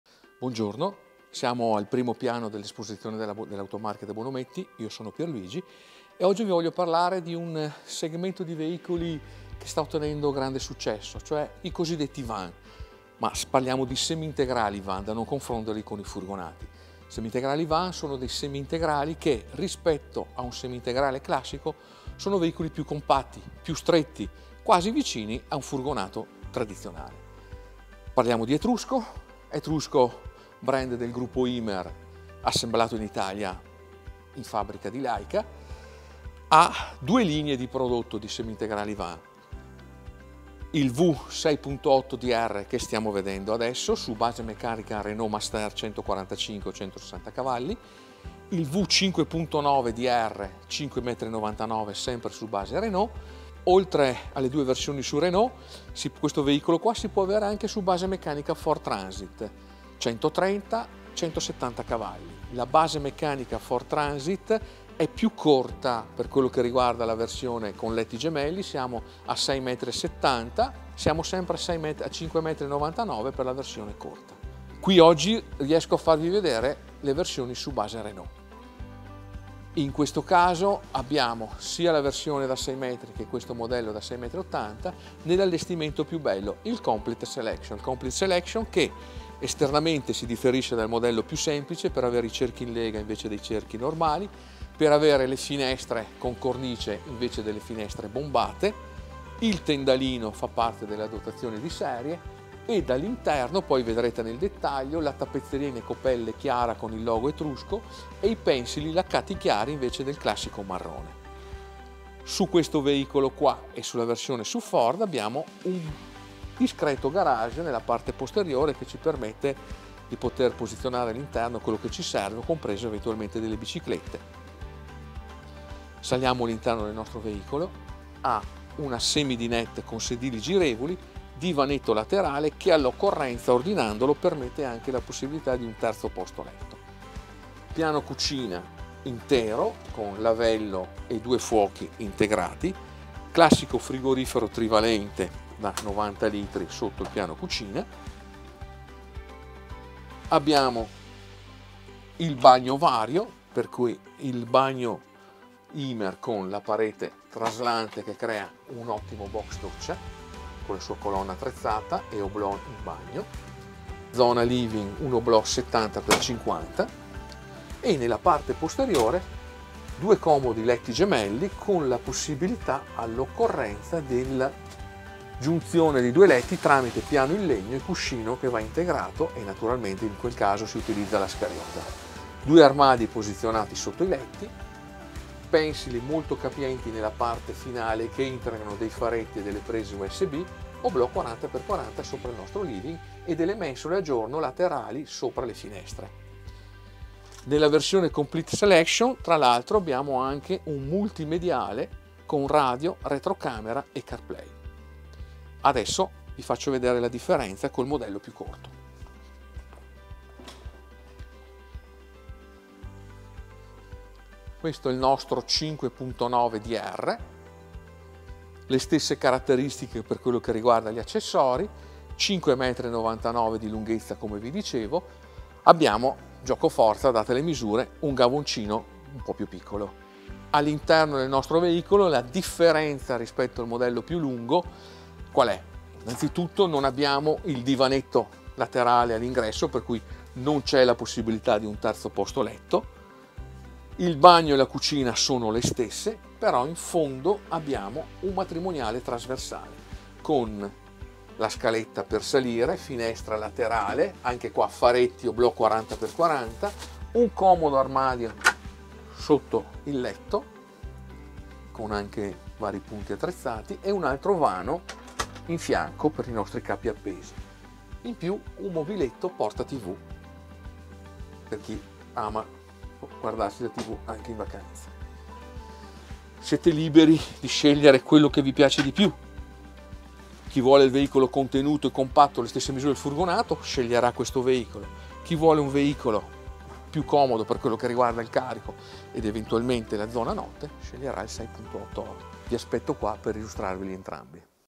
Vi presentiamo Etrusco V 5.9 E 6.8
Oggi vogliamo presentarvi i nuovi Van Camper di Etrusco. Un' ottima soluzione per avere un veicolo agile su strada ma senza rinunciare agli spazi di un Camper.
Su due lunghezze: il 5.9 per chi vuole qualcosa di più compatto e il 6.8 per chi non vuole rinunciare alla vivibilità interna.
Vi aspettiamo da Bonometti per visionarli!